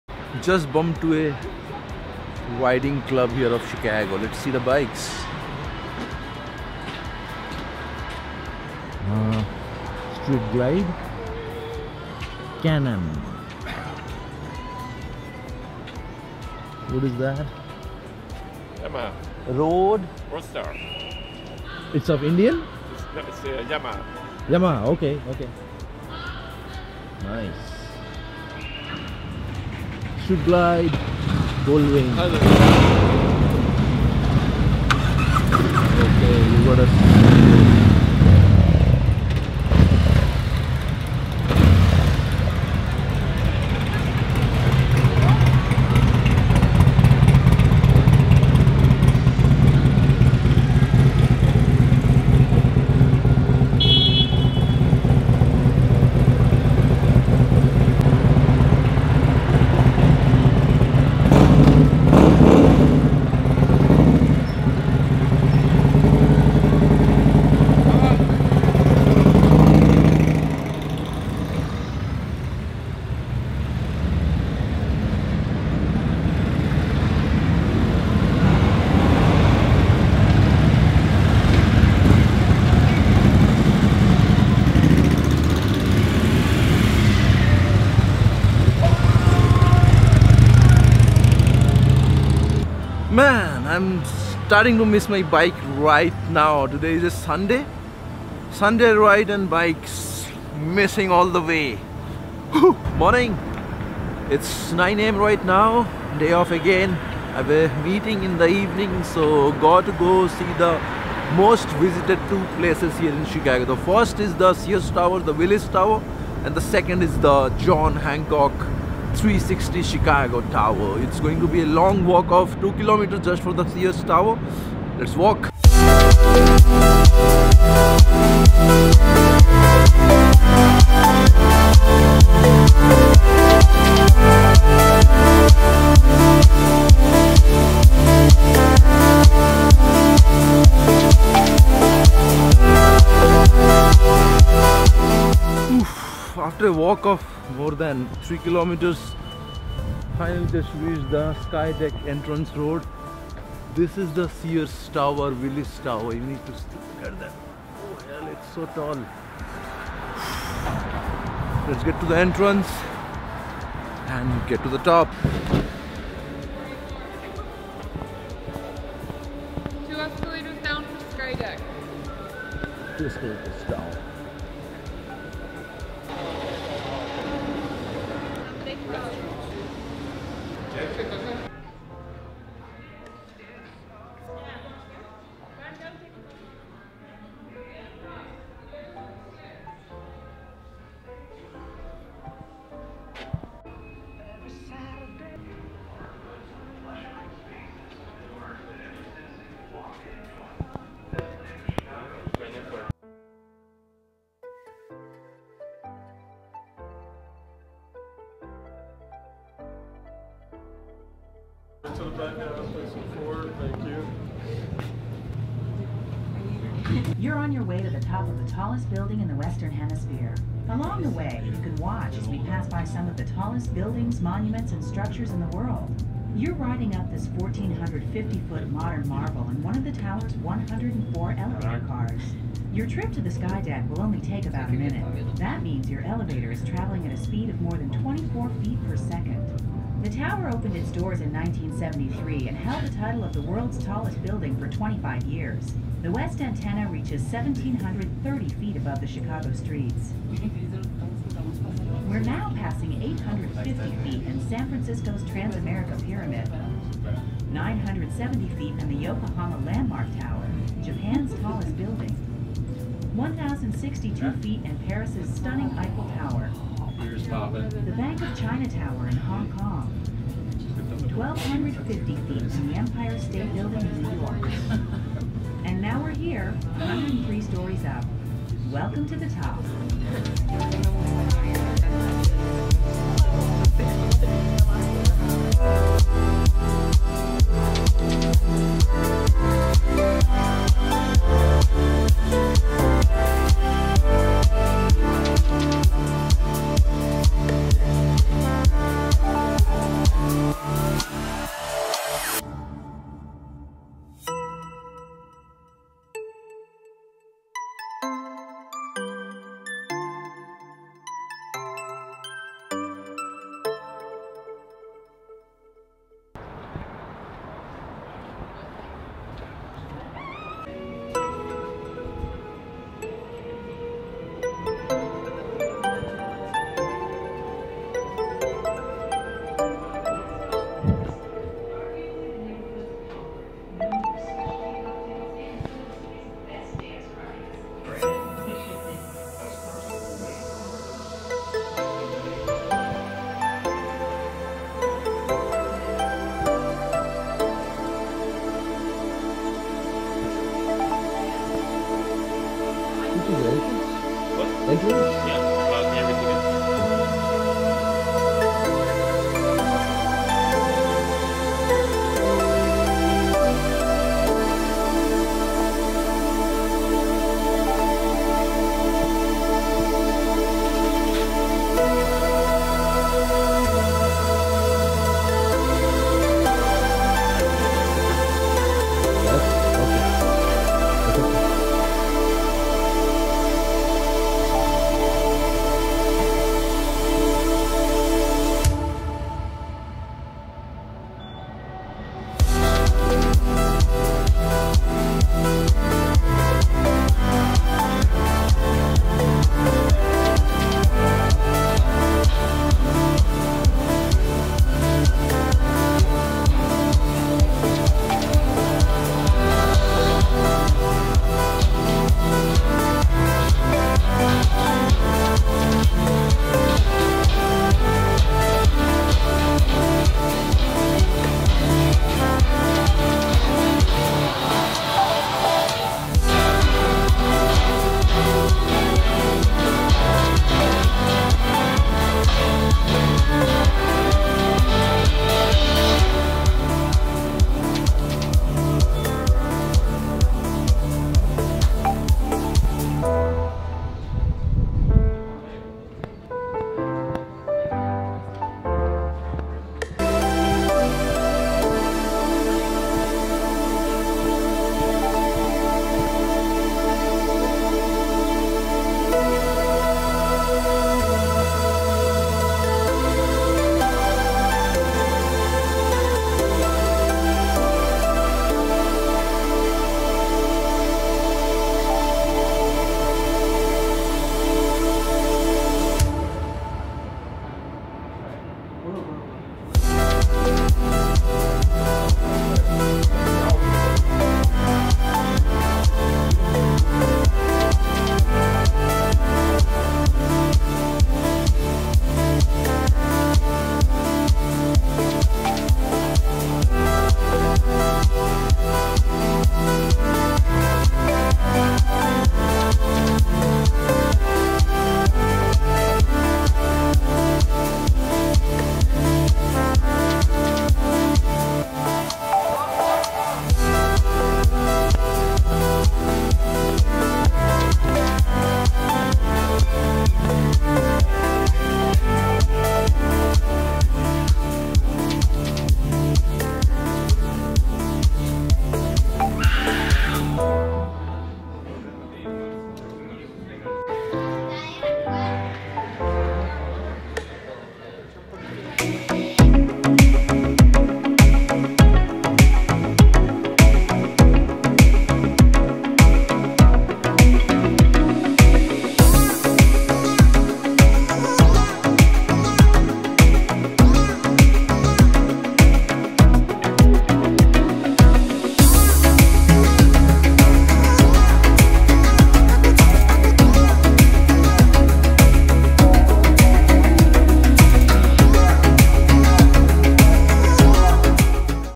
MET THE CHICAGO SUPERBIKERS
So that day I met some of the chicago superbikers and then visited skydeck of Sears Tower (Willis tower) and John Hancock Tower as well.
Chicago is also known as the windy city and its absolutely worth a visit.